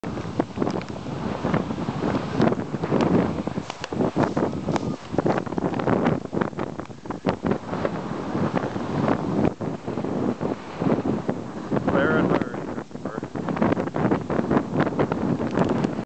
Christopher Ball launches in PEI. Sam was in the air and got up.